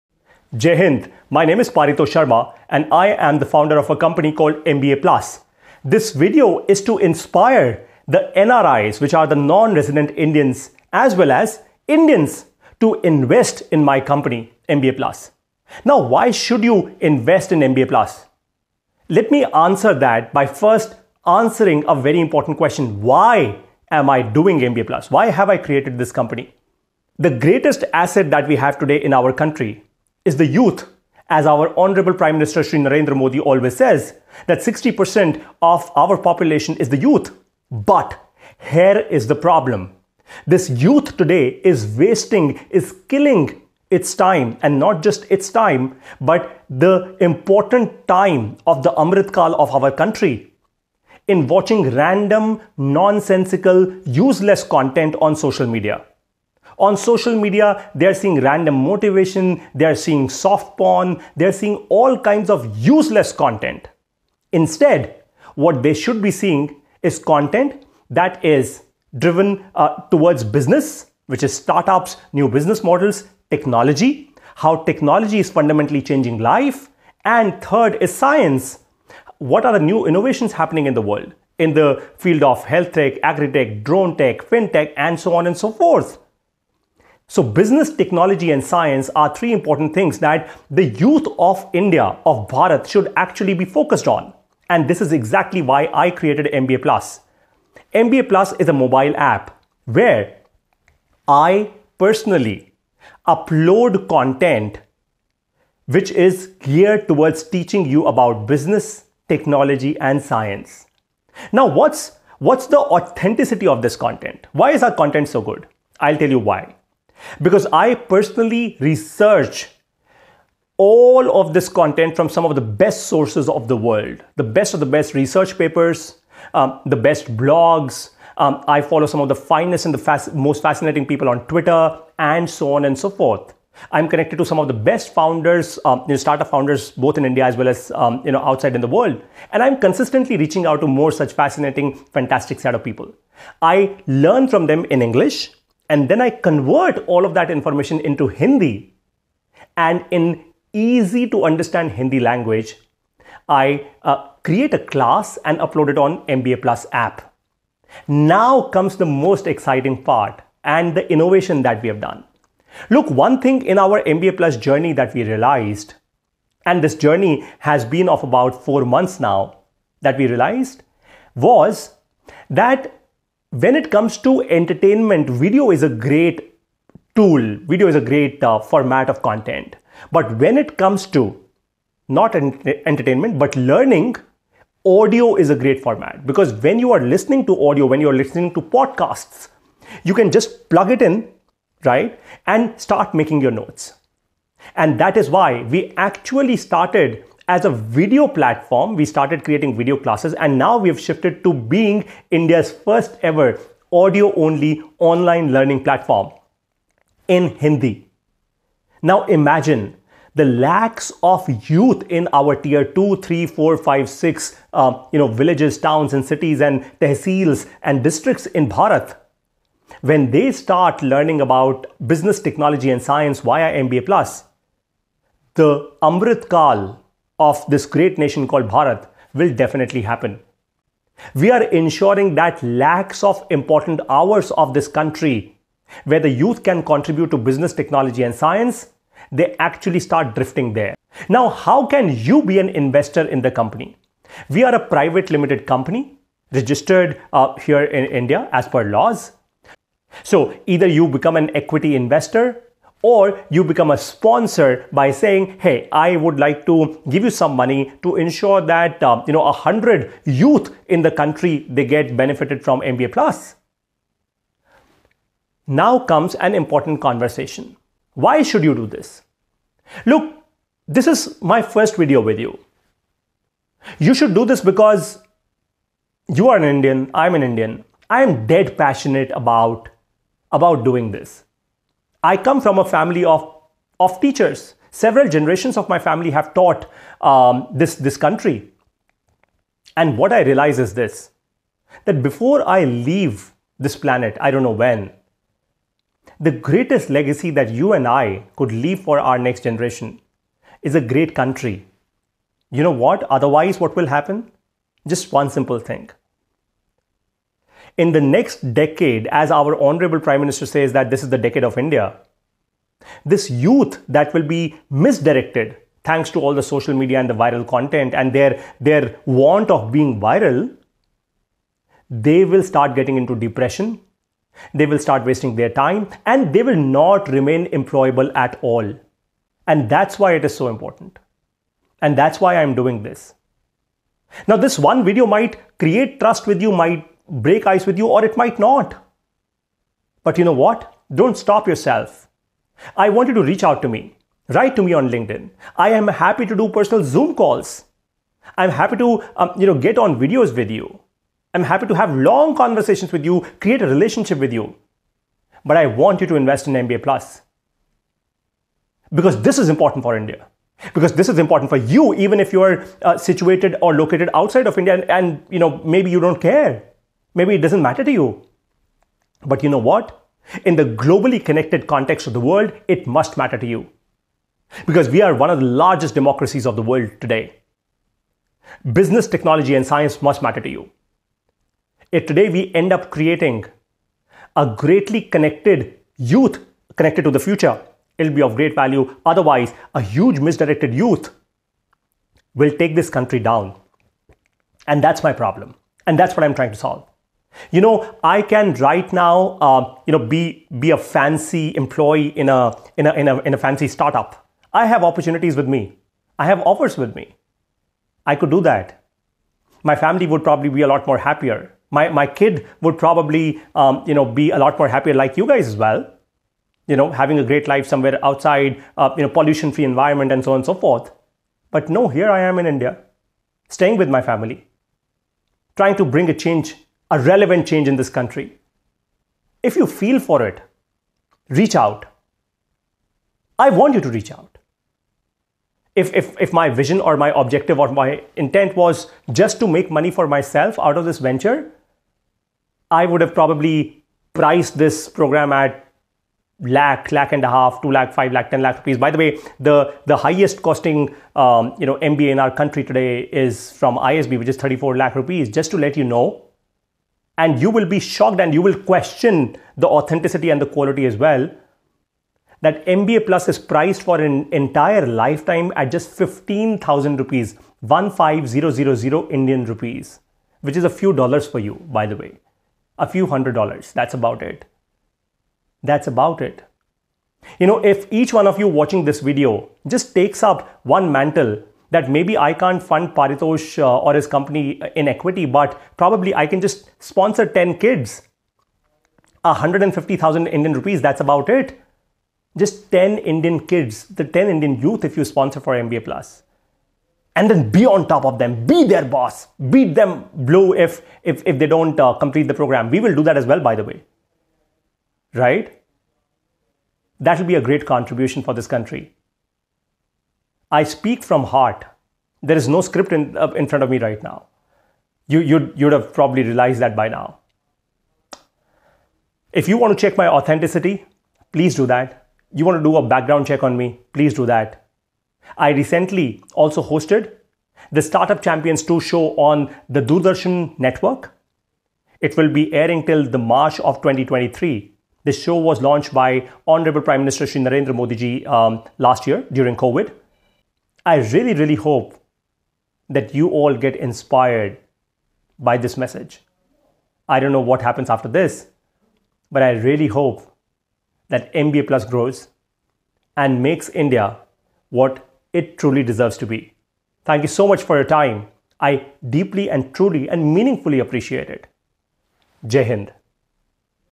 Dear NRIs You Must Act Now For Bharat ! 🇮🇳🙏
Paritosh Sharma, Founder of MBA+ is inspiring NRIs to invest in MBA+